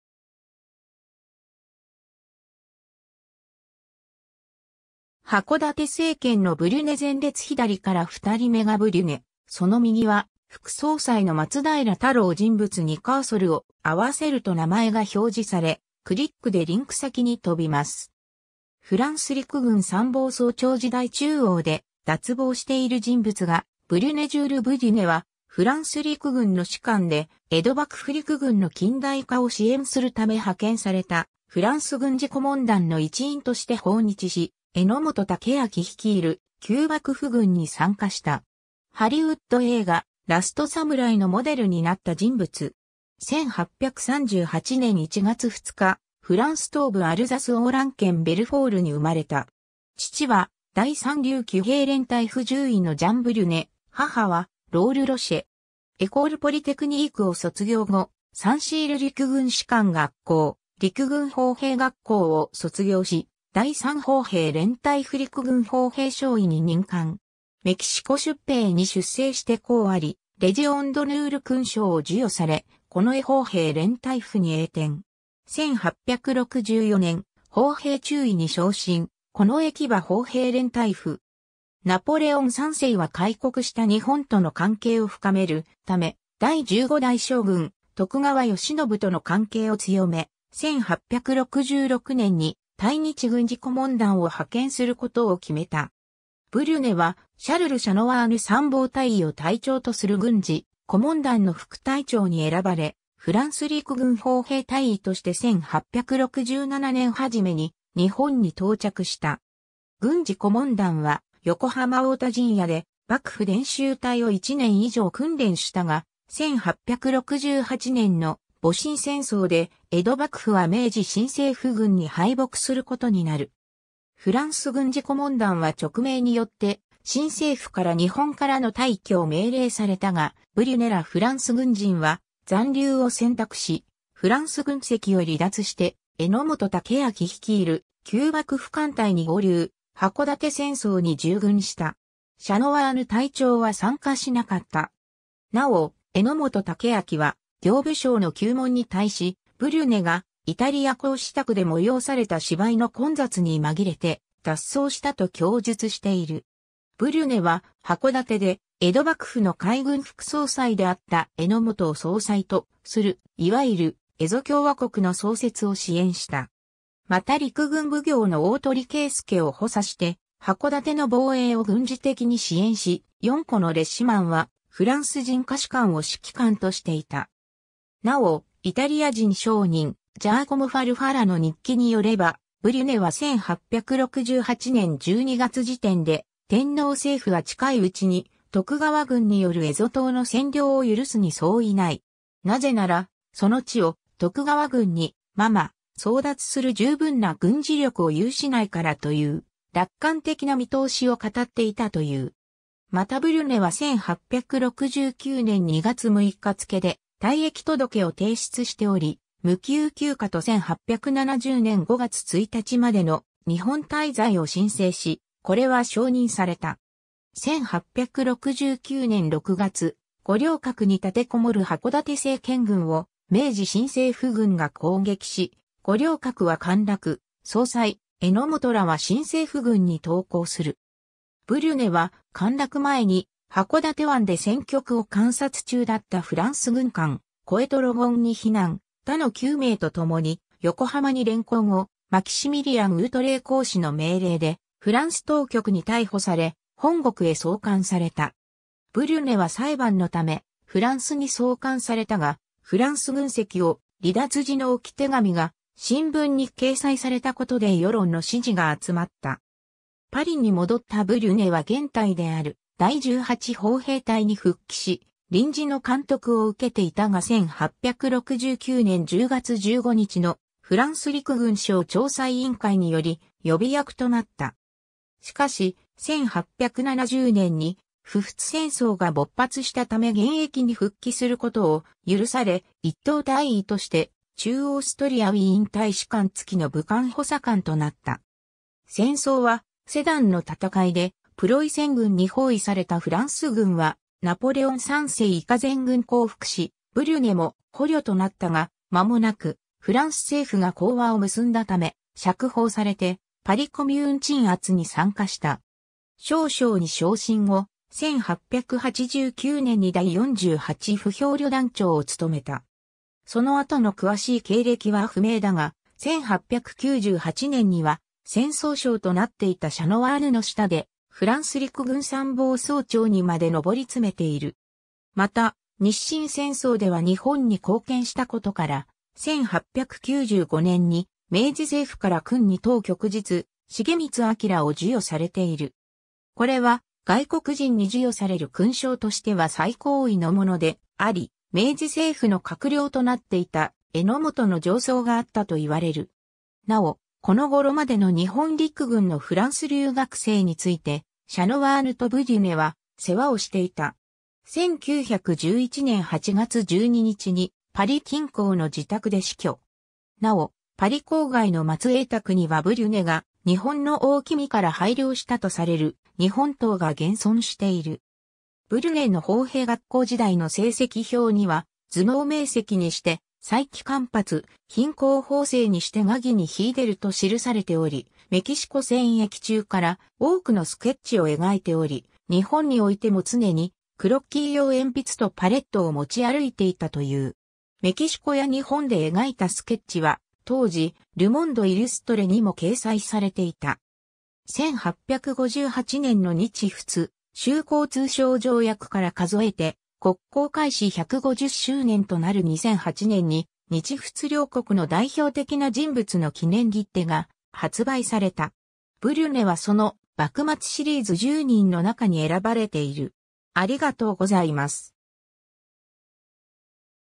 ジュール・ブリュネ
箱館政権のブリュネ（1869年） 前列左から2人目がブリュネ その右は副総裁の松平太郎　- 人物にカーソルを合わせると名前が表示され、クリックでリンク先に飛びます。
 フランス陸軍参謀総長時代（1898年） 中央で脱帽している人物がブリュネ ジュール・ブリュネ（仏: Jules Brunet、1838年1月2日 - 1911年8月12日）は、フランス陸軍の士官で、江戸幕府陸軍の近代化を支援するため派遣されたフランス軍事顧問団の一員として訪日し、榎本武揚率いる旧幕府軍に参加した。
ハリウッド映画『ラスト サムライ』のモデルになった人物。
 1838年1月2日、フランス東部アルザス・オー=ラン県ベルフォールに生まれた。
父は第3竜騎兵連隊附獣医のジャン・ブリュネ、母はロール・ロシェ。
エコール・ポリテクニーク（理工科学校）を卒業後、サン・シール陸軍士官学校、陸軍砲兵学校を卒業し、第3砲兵連隊附陸軍砲兵少尉に任官。
メキシコ出兵に出征して功あり、レジオンドヌール勲章を授与され、近衛砲兵連隊附に栄転。
1864年、砲兵中尉に昇進、近衛騎馬砲兵連隊附。
 ナポレオン3世は開国した日本との関係を深めるため、第15代将軍・徳川慶喜との関係を強め、1866年に対日軍事顧問団を派遣することを決めた。
ブリュネはシャルル・シャノワーヌ参謀大尉を隊長とする軍事顧問団の副隊長に選ばれ、フランス陸軍砲兵大尉として1867年初めに日本に到着した。
軍事顧問団は横浜大田陣屋で幕府伝習隊を1年以上訓練したが、1868年の戊辰戦争で江戸幕府は明治新政府軍に敗北することになる。
フランス軍事顧問団は勅命によって新政府から日本からの退去を命令されたが、ブリュネらフランス軍人は残留を選択し、フランス軍籍を離脱して榎本武揚率いる旧幕府艦隊に合流、箱館戦争に従軍した。
シャノワーヌ隊長は参加しなかった。
なお、榎本武揚は刑部省の糺問に対し、ブリュネがイタリア公使宅で催された芝居の混雑に紛れて脱走した（芝居相催其混雜ニ紛レ脱シ候由）と供述している。
 ブリュネは、箱館で江戸幕府の海軍副総裁であった榎本を総裁とする、いわゆる「蝦夷共和国」（箱館政権）の創設を支援した。
また陸軍奉行の大鳥圭介を補佐して箱館の防衛を軍事的に支援し、4個の列士満（レジマン、フランス語で連隊を意味する "régiment" をそのまま当て字にした）はフランス人下士官（フォルタン、マルラン、カズヌーヴ、ブッフィエ）を指揮官としていた。
なお、イタリア人商人、ジャーコモ・ファルファラ（Giacomo Farfara）の日記によれば、ブリュネは1868年12月時点で「天皇政府は近いうちに徳川軍による蝦夷島の占領を許すに相違ない。なぜなら、その地を徳川軍に〔ママ〕争奪する十分な軍事力を有しないから」という楽観的な見通しを語っていたという。
またブリュネは1869年2月6日付けで退役届けを提出しており、無給休暇と1870年5月1日までの日本滞在を申請し、これは承認された。
 1869年6月、五稜郭に立て籠もる箱館政権軍を明治新政府軍が攻撃し、五稜郭は陥落、総裁・榎本らは新政府軍に投降する。
ブリュネは陥落前に箱館湾で戦局を観察中だったフランス軍艦コエトロゴンに避難、他の9名とともに横浜に連行後、マキシミリアン・ウートレー公使の命令でフランス当局に逮捕され、本国へ送還された。
 ブリュネは裁判のためフランスに送還されたが、フランス軍籍を離脱時の置き手紙が新聞に掲載されたことで世論の支持が集まった。
パリに戻ったブリュネは原隊である第18砲兵隊に復帰し、臨時の監督を受けていたが1869年10月15日のフランス陸軍省調査委員会により予備役となった。
しかし、1870年に普仏戦争が勃発したため現役に復帰することを許され、一等大尉として駐オーストリア・ウィーン大使館付きの武官補佐官となった。
戦争はセダンの戦いでプロイセン軍に包囲されたフランス軍はナポレオン3世以下全軍降伏し、ブリュネも捕虜となったが、間もなくフランス政府が講和を結んだため、釈放されてパリ・コミューン鎮圧に参加した。
少将に昇進後、1889年に第48歩兵旅団長を務めた。
その後の詳しい経歴は不明だが、1898年には戦争相となっていたシャノワーヌの下でフランス陸軍参謀総長にまで登りつめている。
 ま...